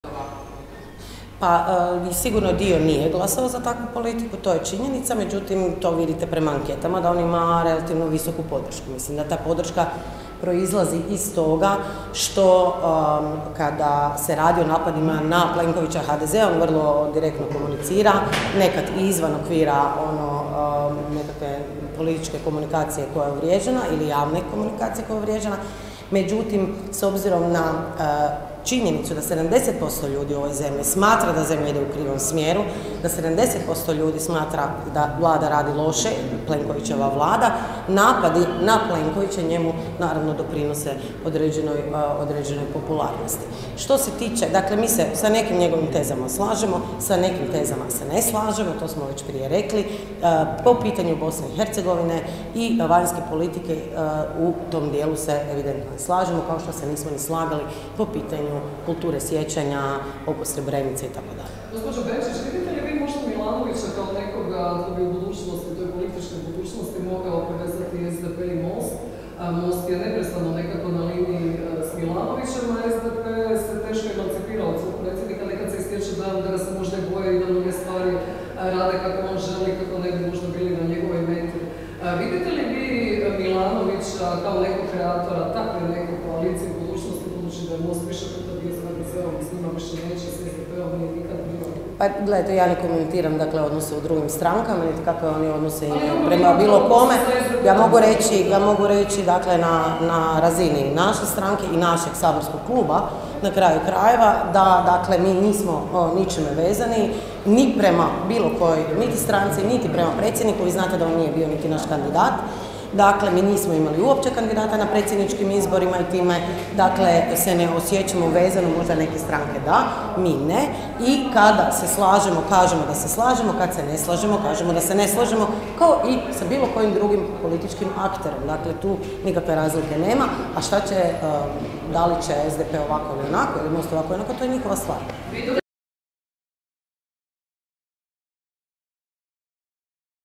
Benčić: Milanović po anketama i dalje ima relativno visoku podršku
Na konferenciji za medije održanoj u Saboru zastupnica Sandra Benčić komentirala je aktualna događanja.